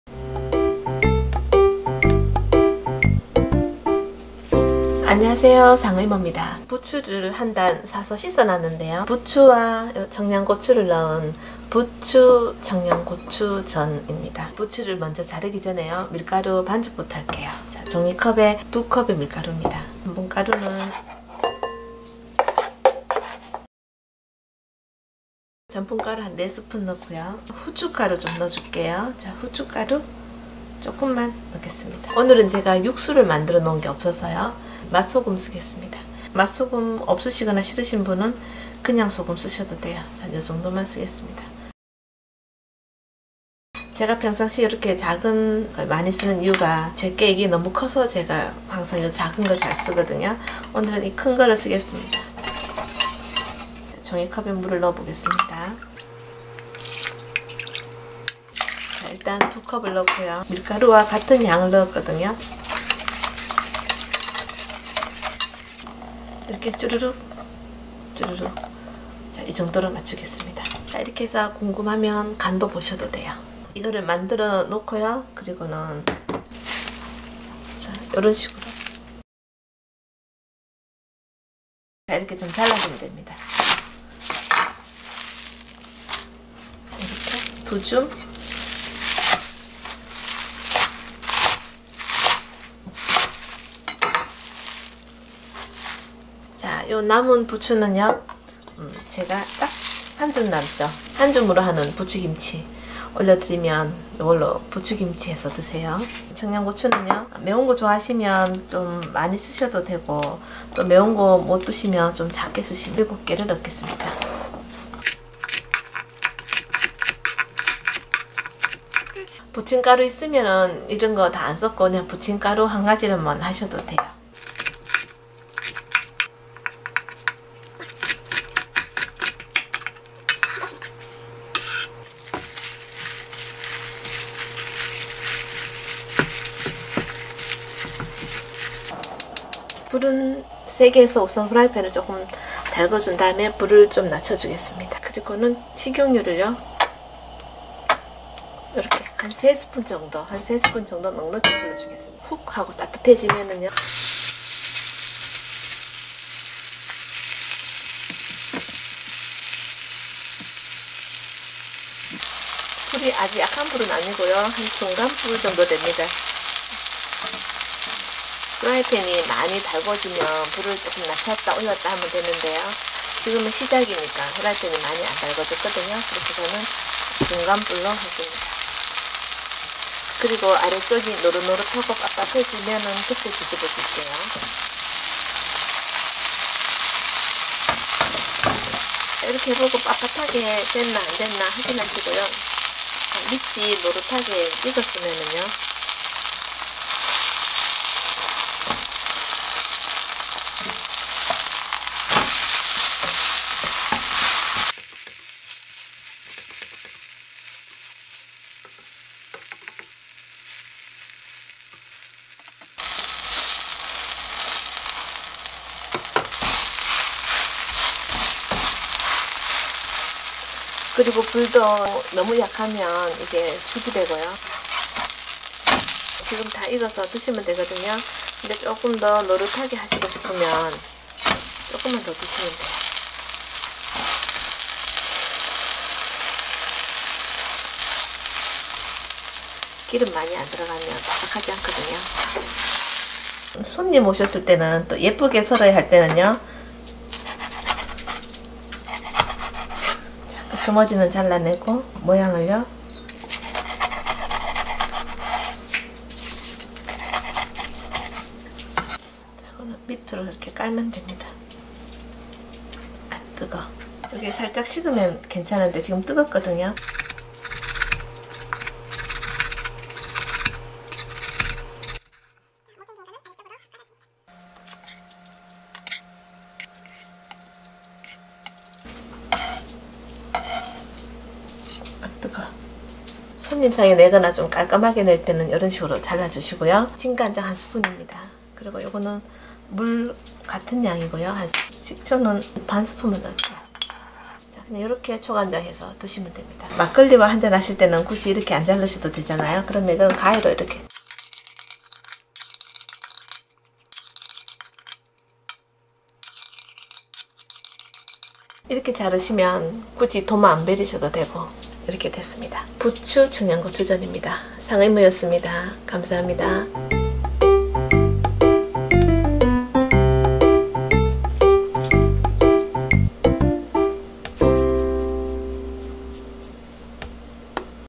부추전과 청양고추의매콤한 만남 땡초부추전/비오는날/땡초정구지찌짐
오늘은 매콤한 풋고추를 넣어서 전을구워봤습니다 .
상d어이모네이버TV
상어이모유튜브

부추전재료
부추2줌/청양고추/
밀가루2종이컵/물2종이컵/전분가루4스푼/소금약간(맛소금)